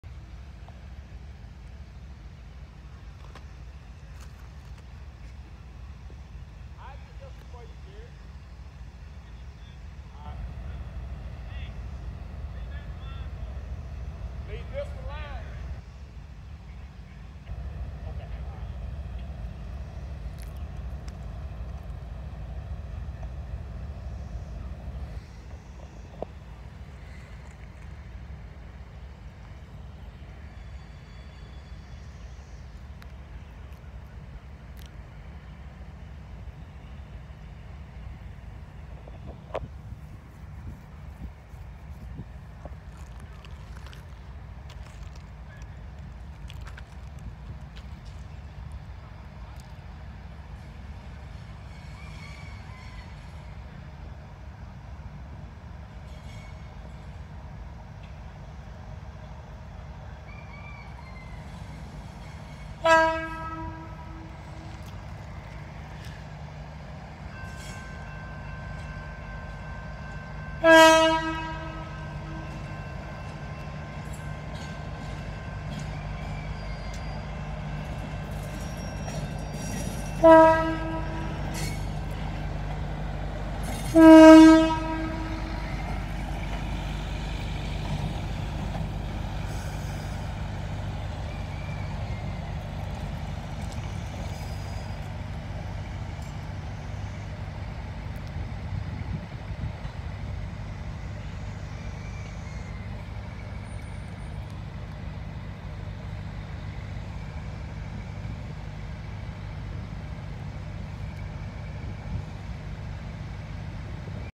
SRS 145 heads off for a track inspection. 5-13-24
So on this cloudy Monday, this was all that was going on in greenwood. No the yard didn’t start working until after I left 6 minutes later

Copyright @csxtrainfan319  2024 don’t use without permission